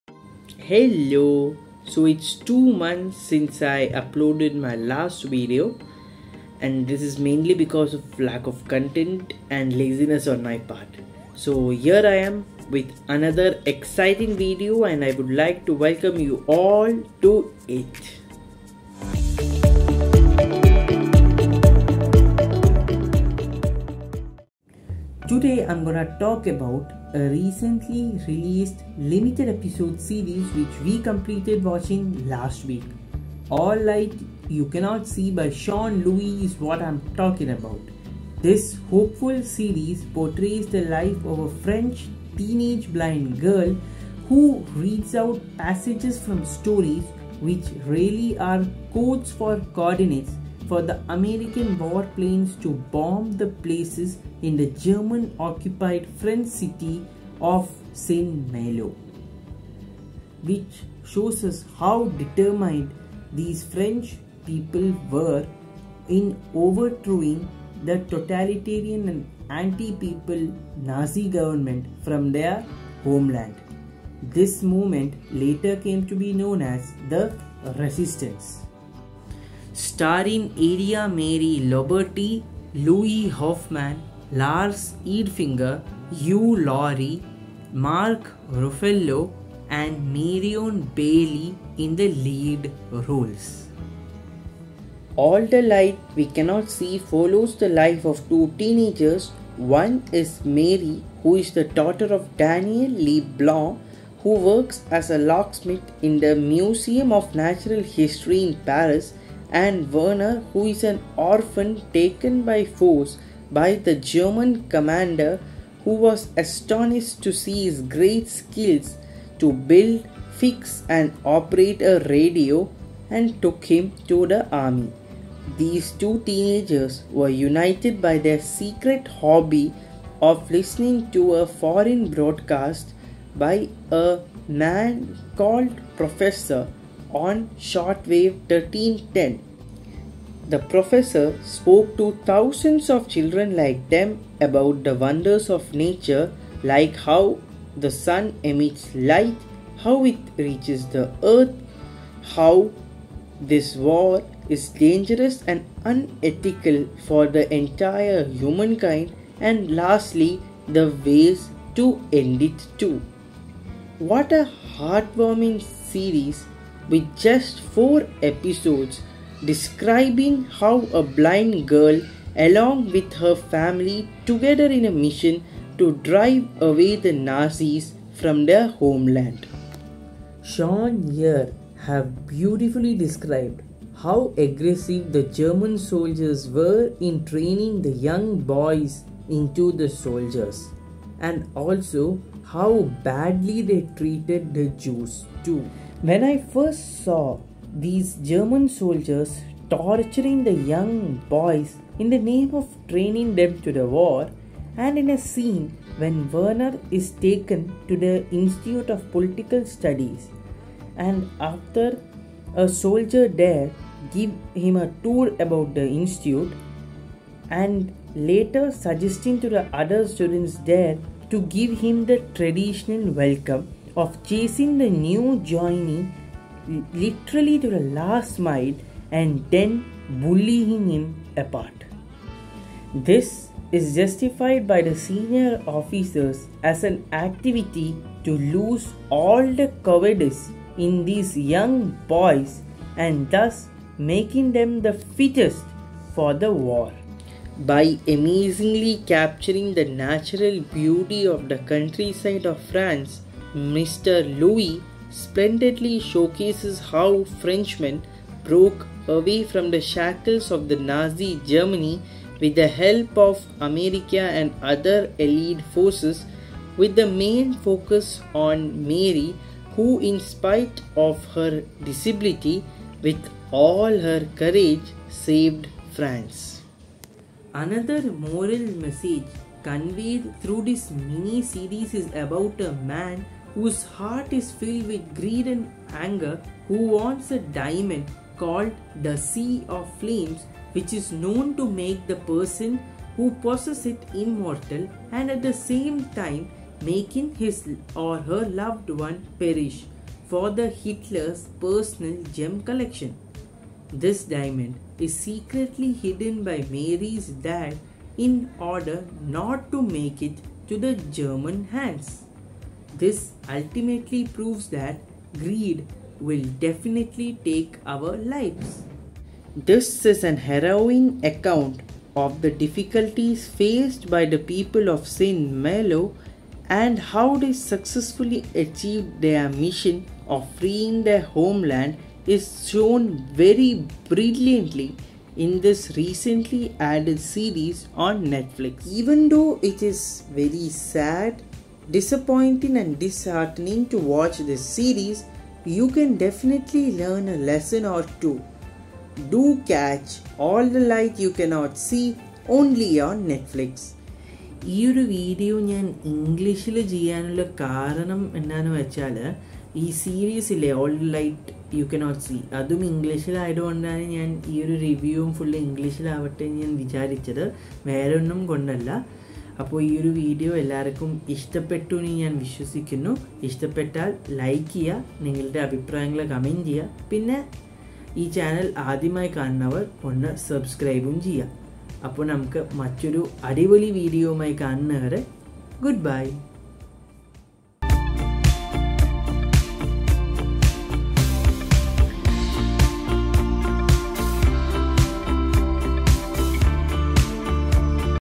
All The Light We Cannot See Series Review | Only on Netflix | Travel and Leisure Vlog-19
Hello guys, I'm back with a new video after a very looong time.

In this video, I am reviewing the recently added limited episode series All The Light We Cannot See on Netflix. Hope you like this video!